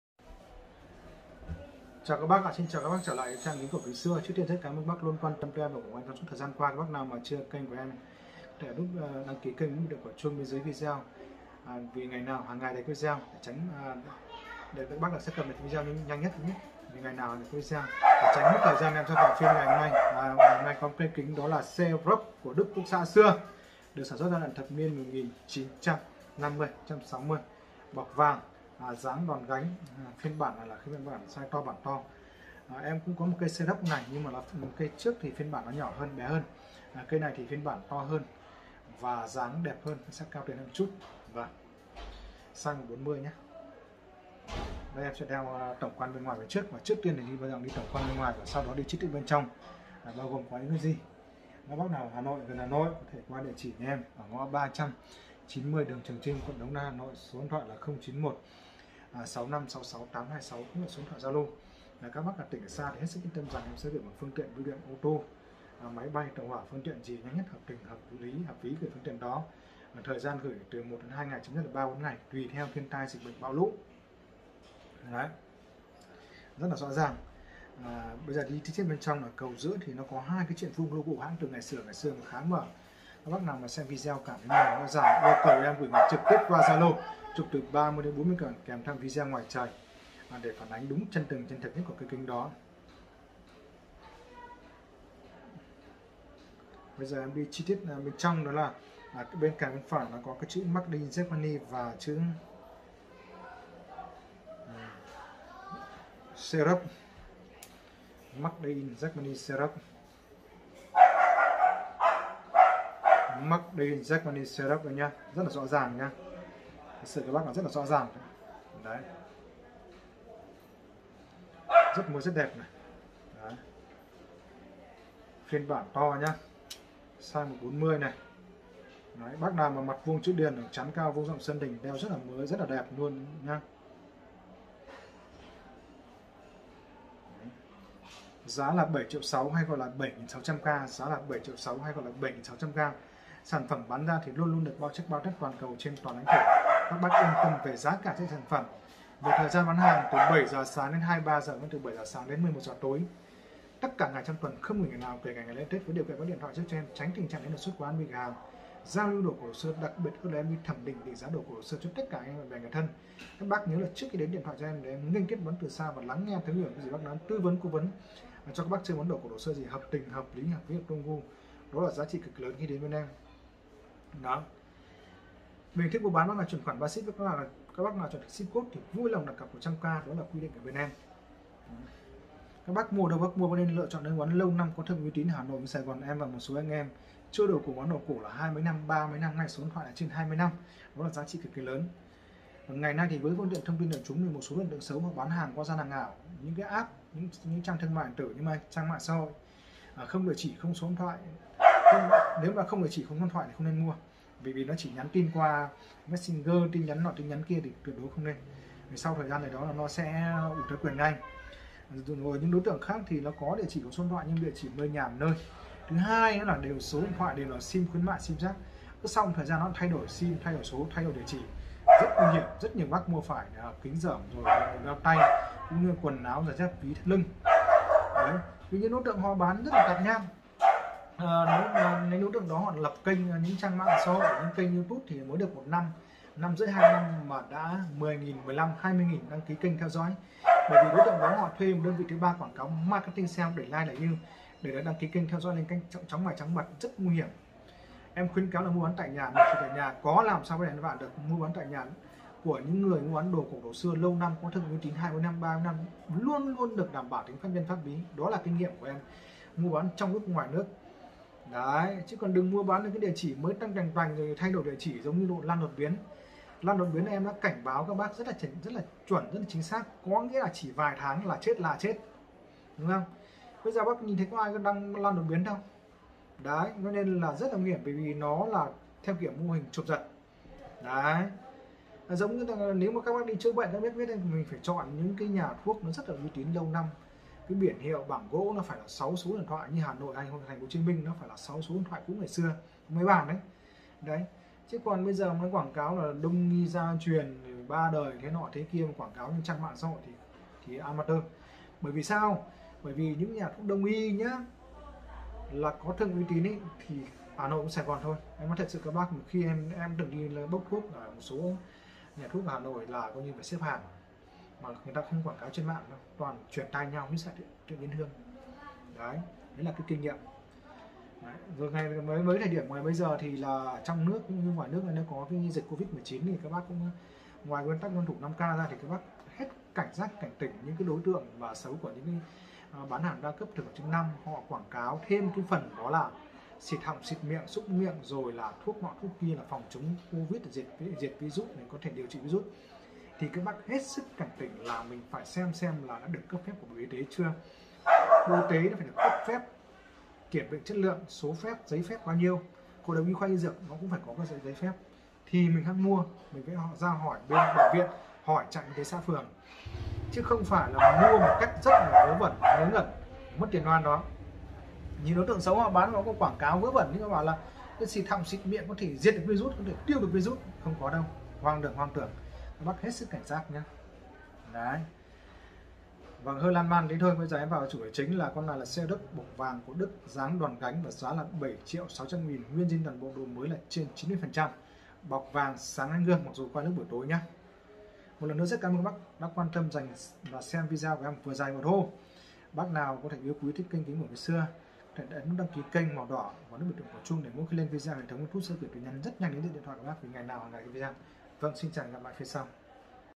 Phá giá kính đòn gánh bọc vàng của Đức Quốc Xã xưa. Jobex Gold Filled.
# Chuyên mua bán kính cổ, kính xưa, đồ cổ, đồ xưa.
Giao lưu kính Jobex đòn gánh bọc vàng của Pháp xưa.
Size: 140.
Kính bọc vàng sx thập niên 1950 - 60.
Kính nguyên zin toàn bộ độ mới trên 94%.
Giá: 7,6tr
+ Hình thức mua bán:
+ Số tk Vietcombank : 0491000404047
- Chủ tk : Lê Anh Việt
- Chi nhánh: Thăng Long – Hà Nội

+ Bác nào chọn hình thức Ship Cod thì vui lòng đặt cọc 100k.